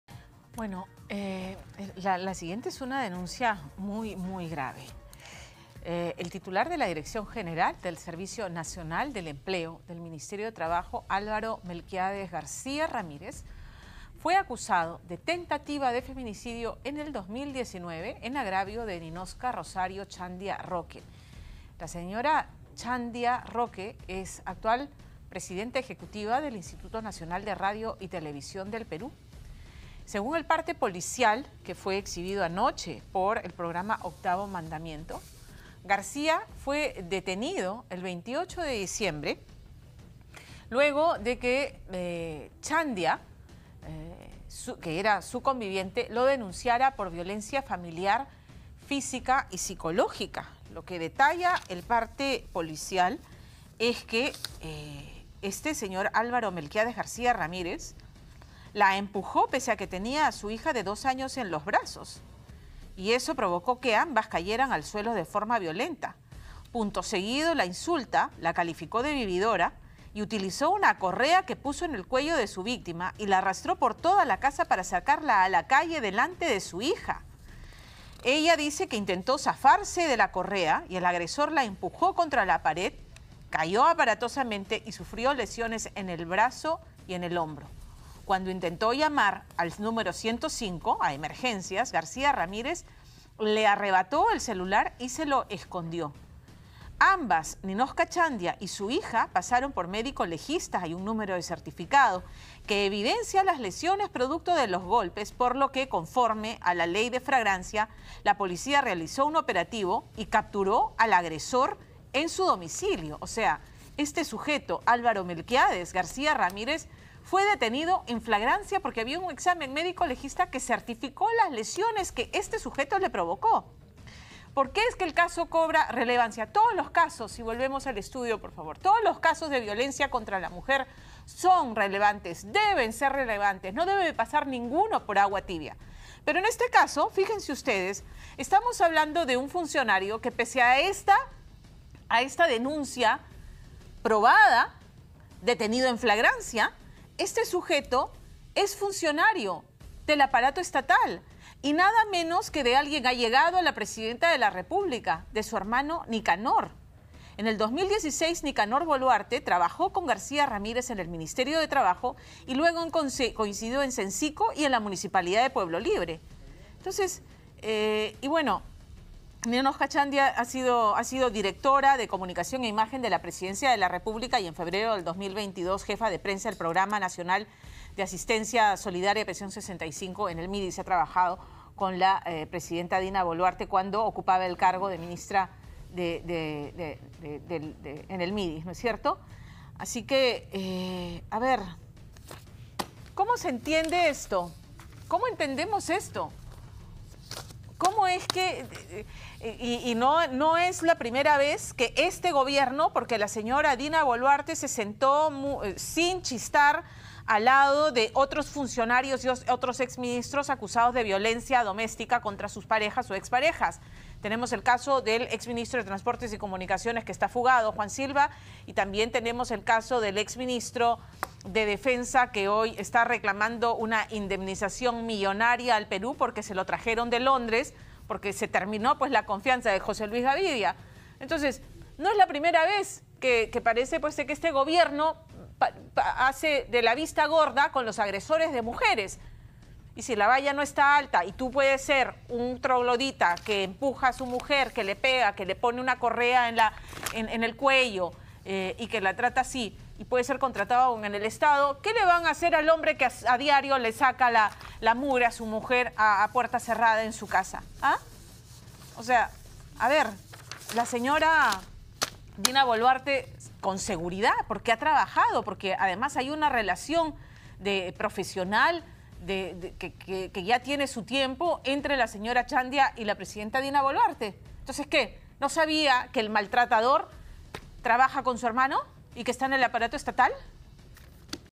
Ministerio de Trabajo: Funcionario es acusado de tentativa de feminicidio contra presidenta del IRTP
En el 2019, Ninoska Chandia denunció a su entonces conviviente por violencia familiar, física y psicológica.